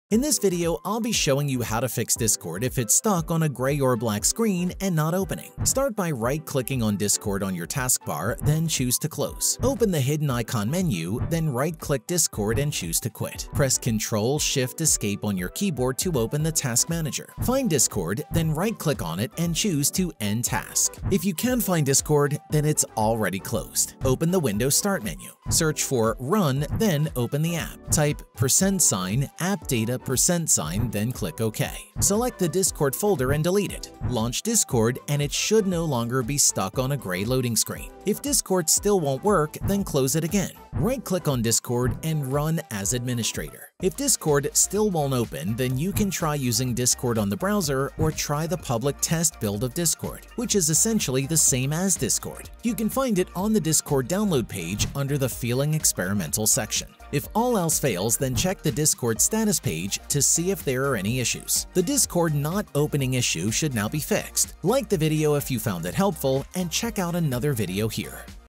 How to Fix Discord Stuck on Grey Screen 2024 - Not Opening
In this tutorial I show you how to fix Discord stuck on grey screen. This issue usually occurs when you're trying to launch Discord resulting in an error where Discord is not opening. To fix the issue you can delete the Discord app data files. Doing so will require Discord to be completely closed. Once the app data folder has been deleted you can open Discord and the needed files will automatically be remade.

Another option is to use an alternate version of Discord. There are two primary choices for this, the browser version and the public test build version. If the Discord stuck on grey screen issue is temporary then you may prefer to use the browser version of Discord temporarily. However, if you want to use an application then the public test build works well as a replacement for the standard Discord app.

Alternatively, the Discord not opening issue may be caused to issues on Discords end. You can check if there are any current issues through the Discord status page.